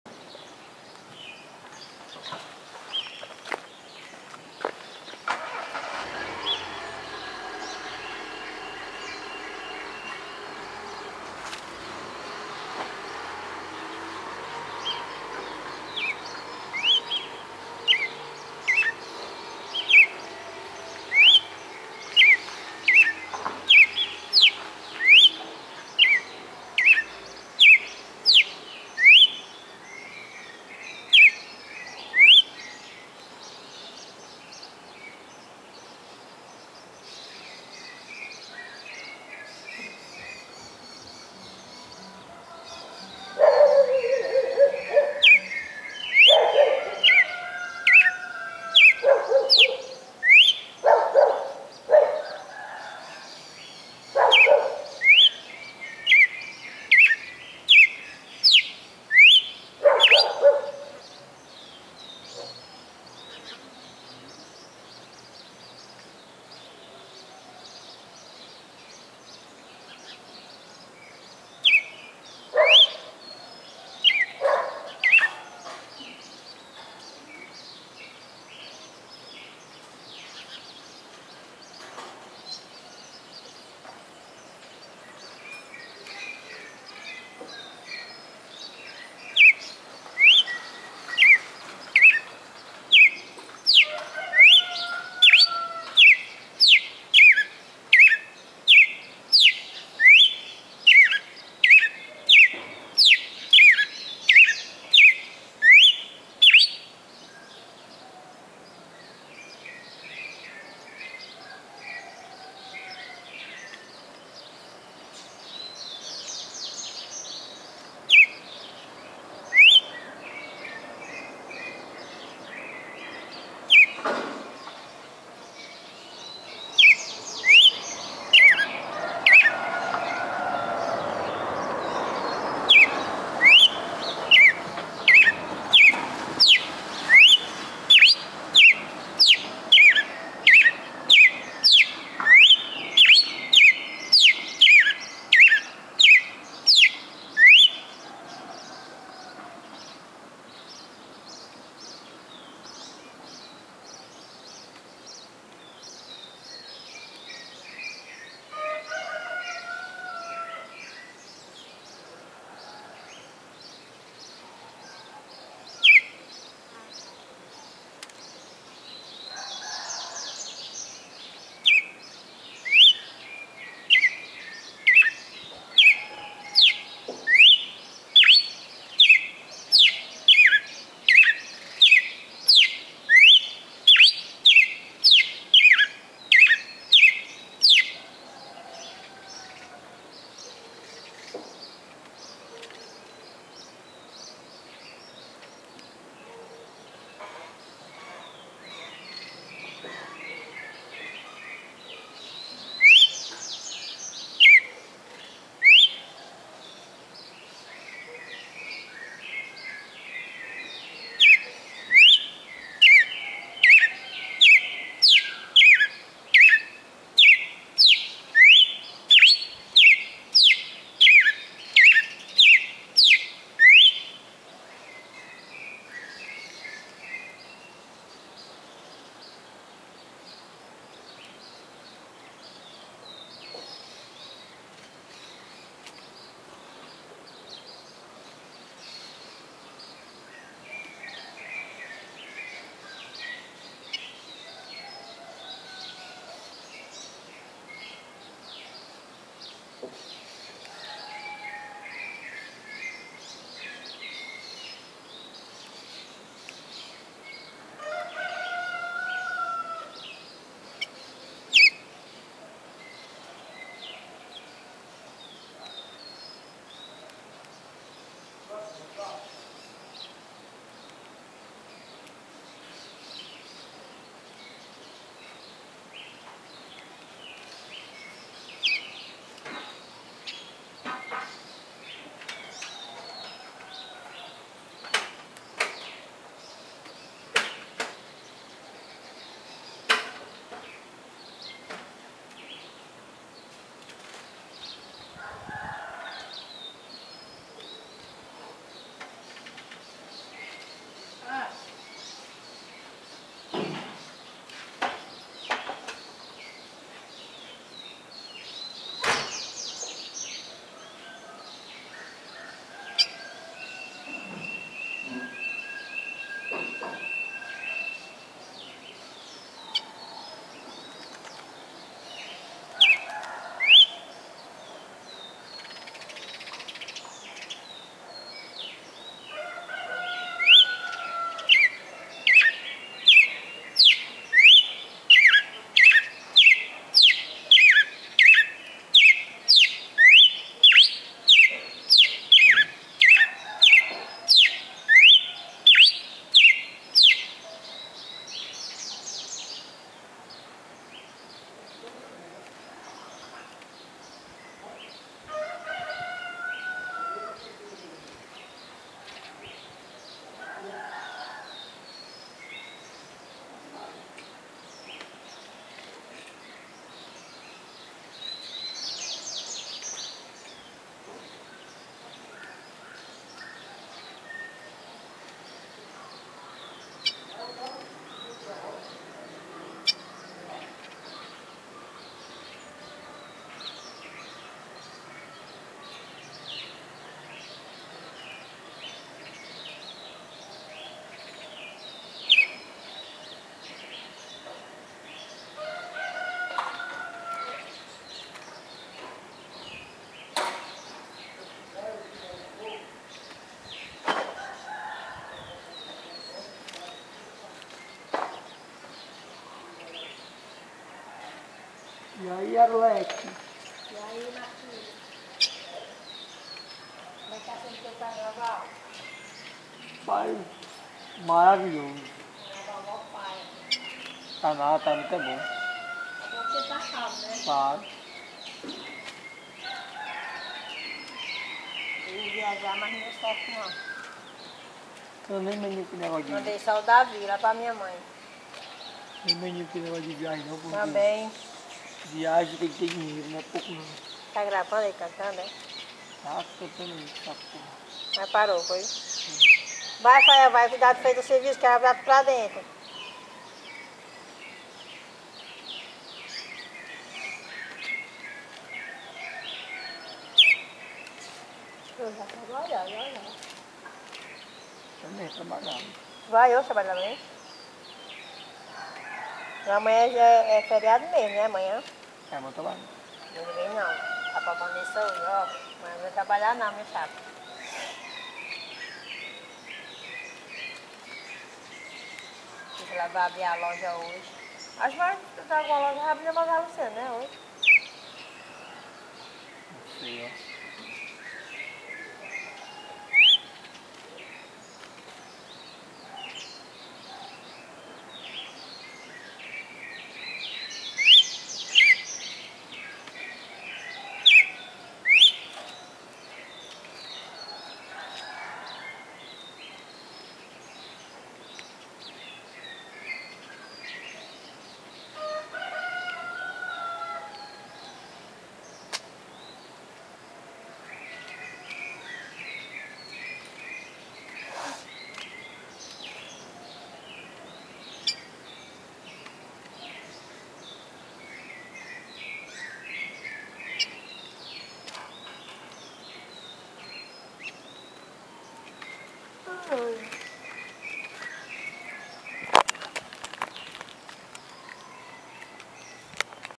Galo de Campina "o canto perfeito" Fortaleza-CE.
Mais de 20 notas perfeitas,foram pouco mais 4 meses que ele ficou comigo,um dos pássaros que mais me deu orgulho com certeza,vai deixar saudades...